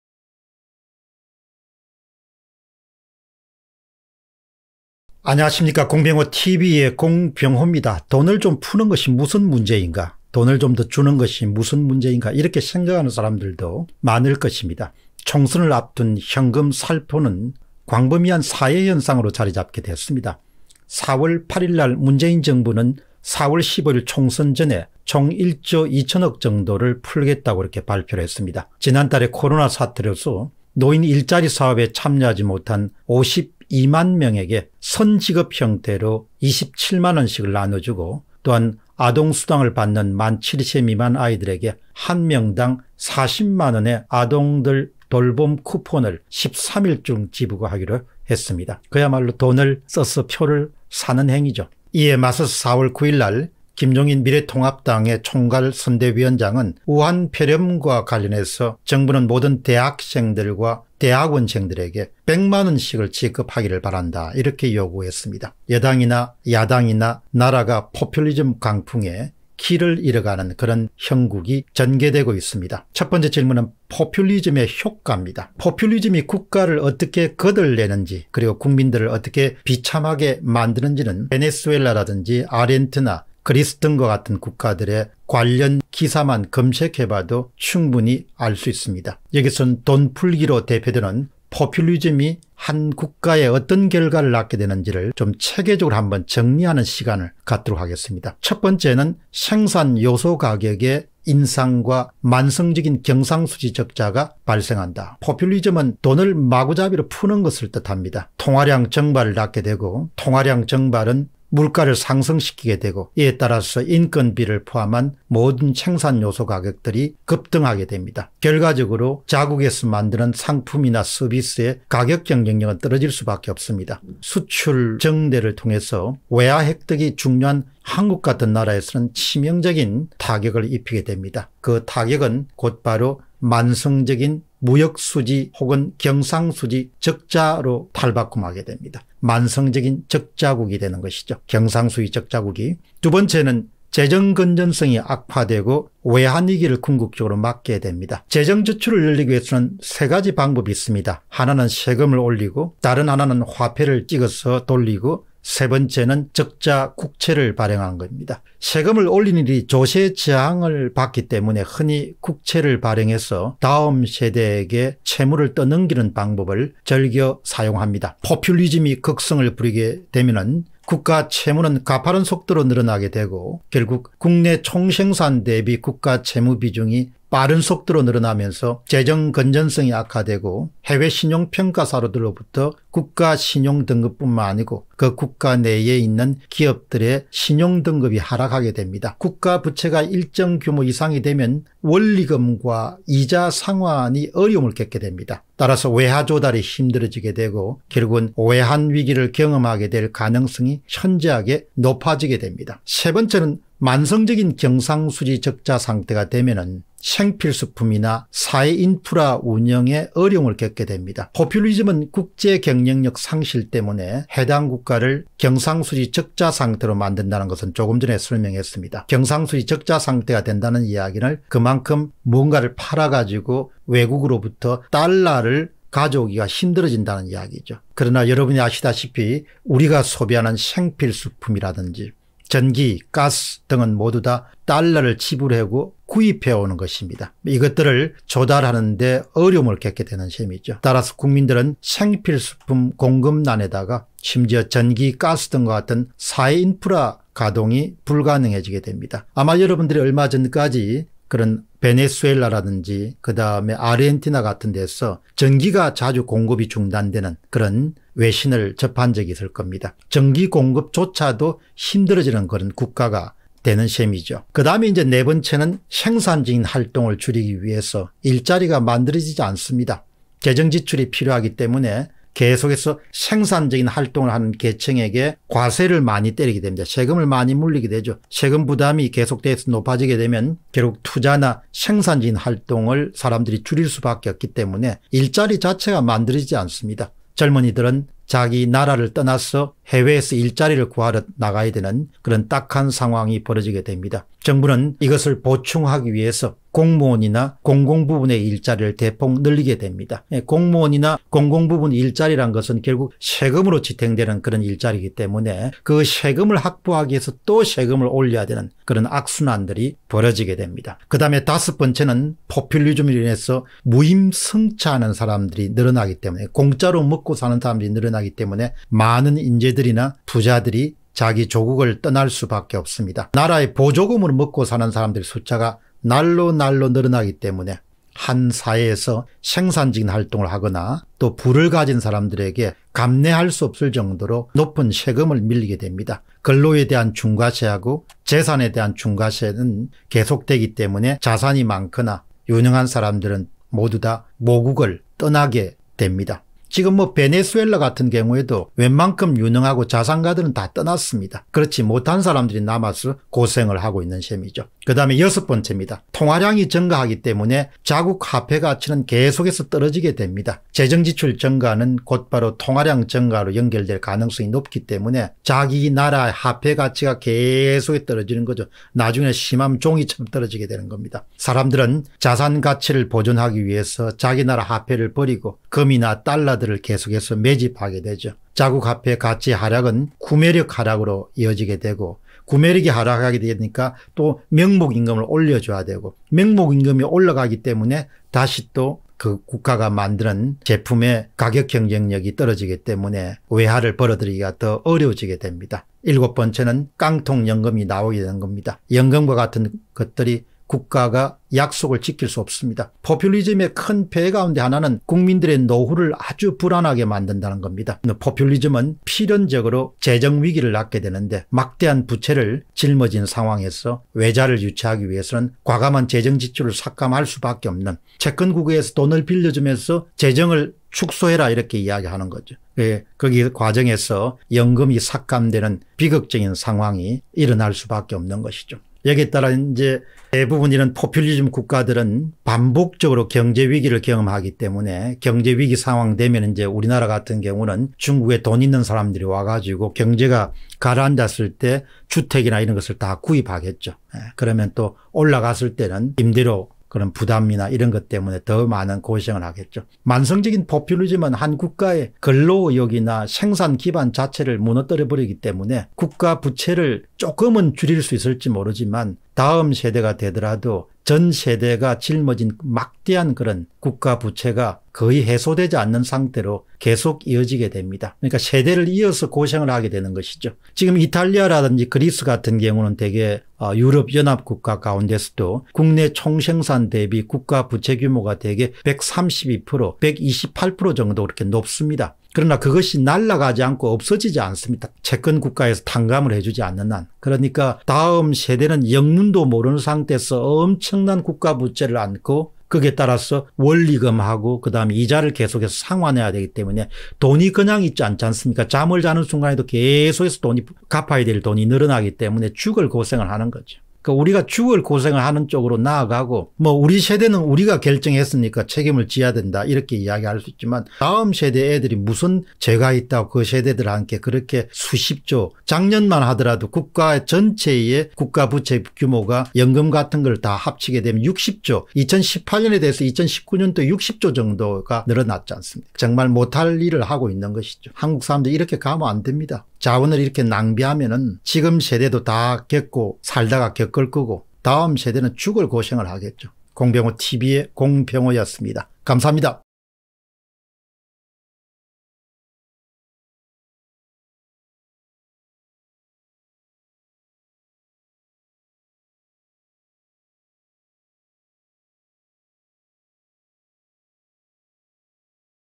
한국의 포퓰리즘 / 이렇게 될 것이다 [공병호TV]
한국의 포퓰리즘 / 이렇게 될 것이다

* 국내 후원하기 :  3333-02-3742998 카카오은행 (예금주 : 공병호)
/ (텔레뱅킹 : 090 & 은행코드 : 90)

참고문헌
1. 노인일자리 임금 선지급, 아동쿠푼, 총선전 1조 몰아치기, 홍준기, 조선일조, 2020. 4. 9
2. [속보]김종인 "모든 대학생ㆍ대학원생에게 100만원씩 지급하라", 최승현, 조선일보, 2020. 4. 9